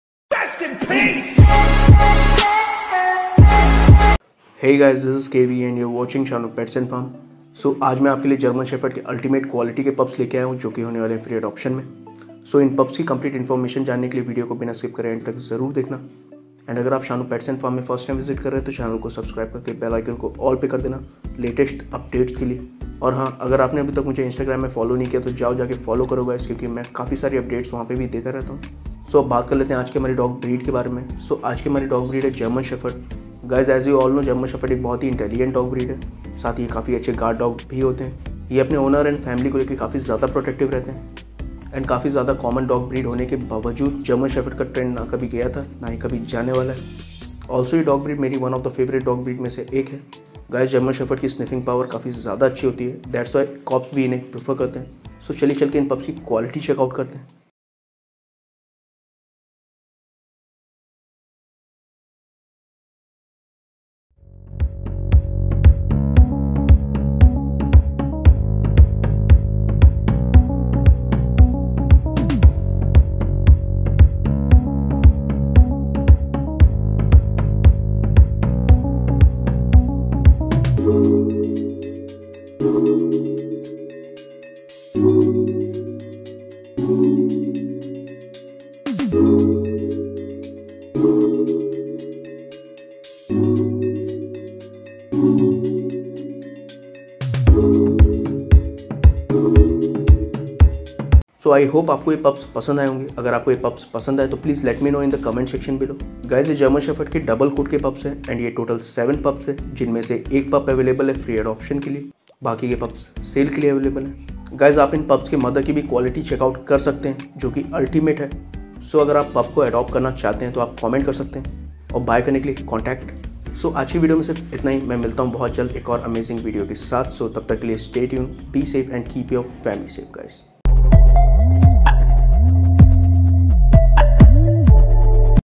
free! free long hair German Shepherd pupps - for free adoption || 2021 || spf
wellcome to shanu's farm

Today's video :long hair Germanshephered for free adoption

Todays breed German Shephered

The German Shepherd is a breed of medium to large-sized working dog that originated in Germany. In the English language, the breed's officially recognized name is German Shepherd Dog.

Life expectancy:

 9 – 13 years

Origin:

Germany

Colors:

Black, Black & Tan, Sable,

Red & Black, Grey,

Black & Silver

Temperament:

Obedient, Intelligent,

Curious, Alert, Loyal,

Confident, Watchful,

Courageous

Height:

Male: 60–65 cm,

Female: 55–60 cm

Weight:

Male: 30–40 kg,

Female: 22–32 kg

please be safe and keep your family safe

shanu farm

thanks to all for this love and support

* All Rights to above Music Lable & No Copyrights

Infringement intended*